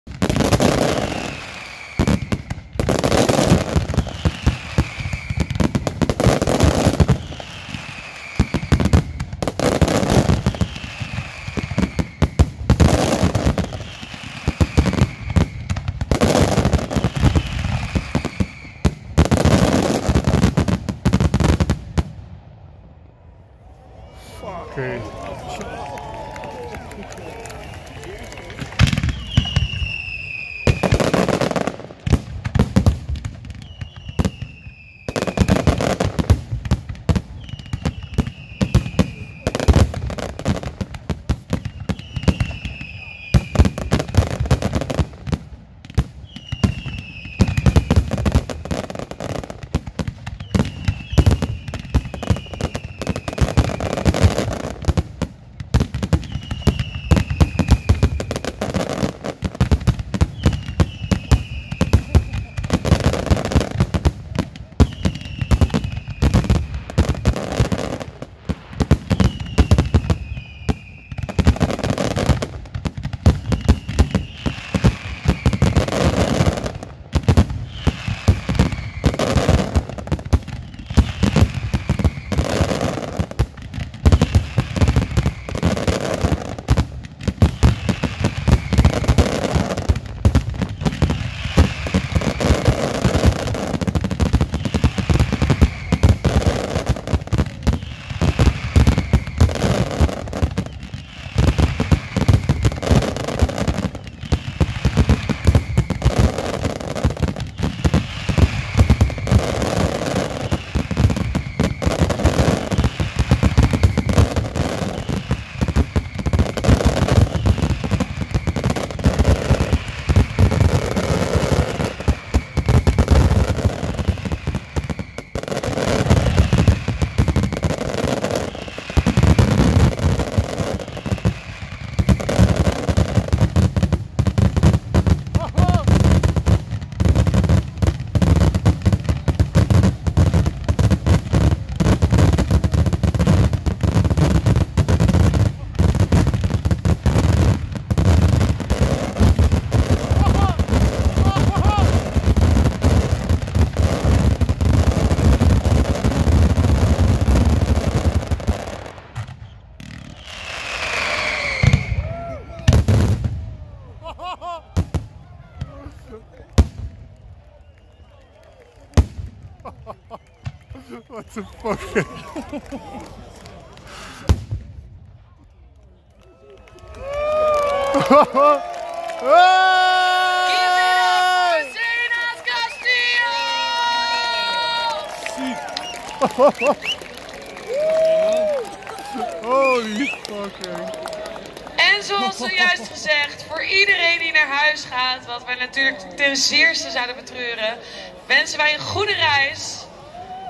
Zena Trophy 2019 (zena's castillo) Deel 2
The grand final wat een geweldig einde van deze editie deel 2. Door een fout van mij zelf ben ik helaas 2 min verloren aan film maar het mooiste staat erop gelukkig.